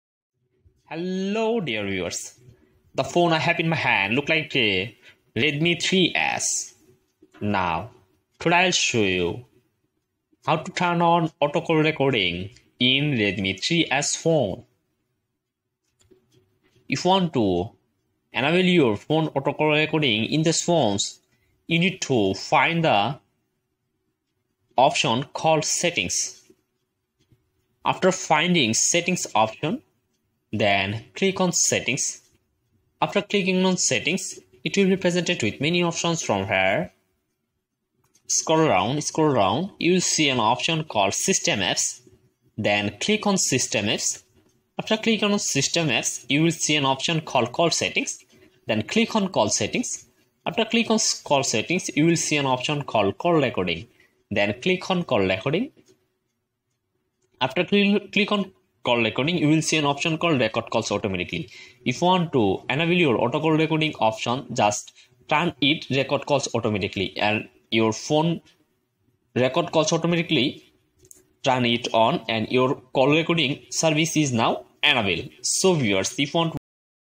How to enable or disable Auto call recording in Redmi 3s
That you can see a phone this phone model is Redmi 3s. now today i will show you how to turn on auto call recording in redmi 3s phone and i will also show you today how to disable auto call recording in redmi 3s phone.